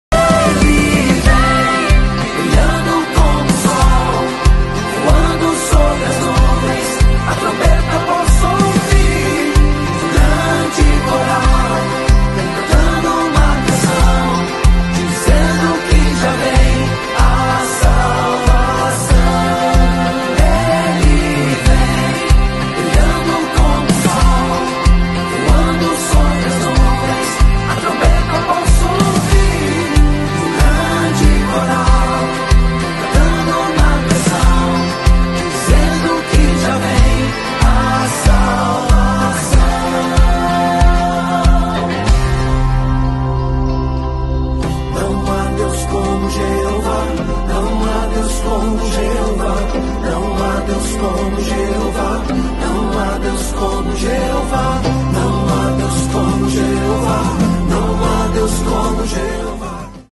1° dia do Congresso de Senhoras | 21° aniversário do Círculo de Oração | IADVC Paraipaba-ce
Vídeo de Bráulio Medeiros